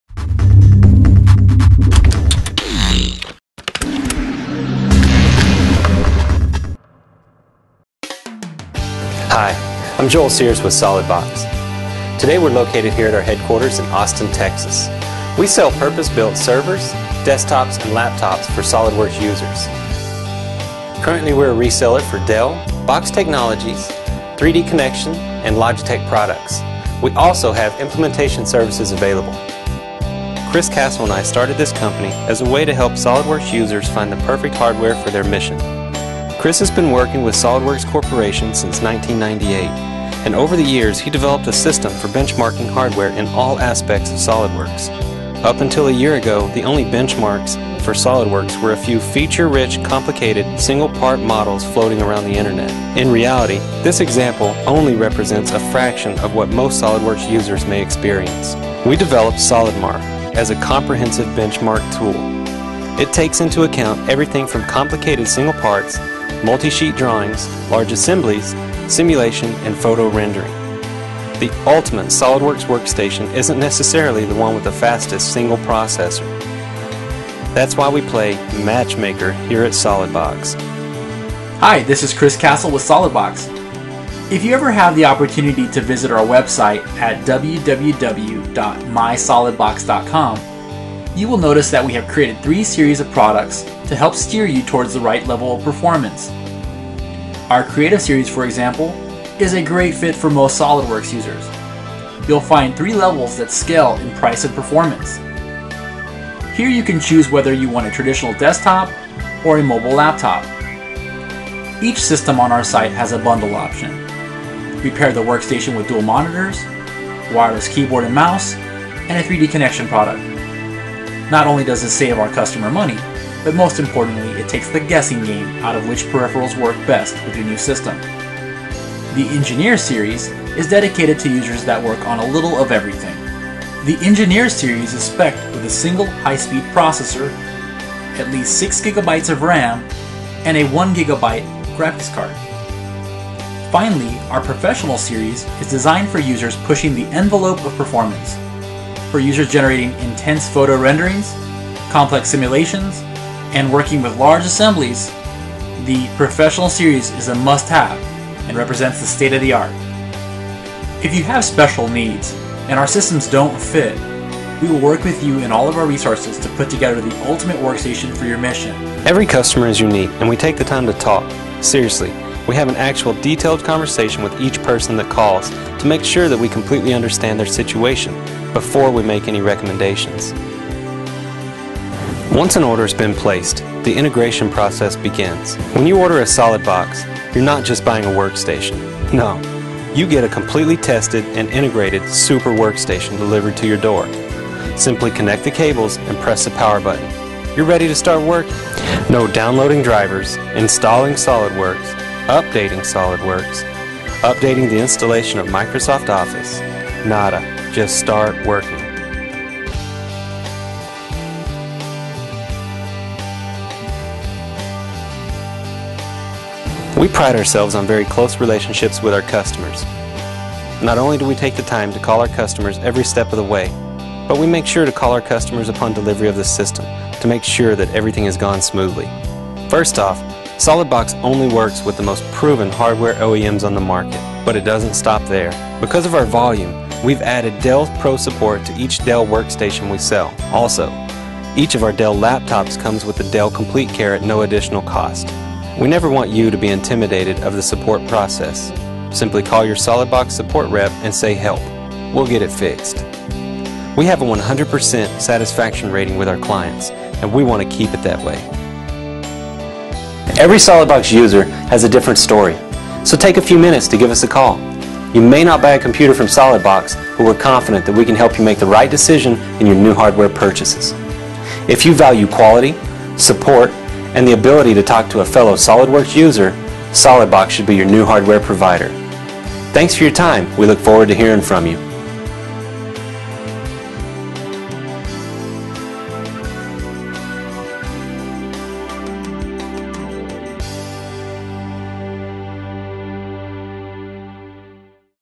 SolidWorks Hardware Solutions
This video explains what SolidBox provides SolidWorks end users. SolidBox Installs SolidWorks and gets the computer ready to work before you open the box!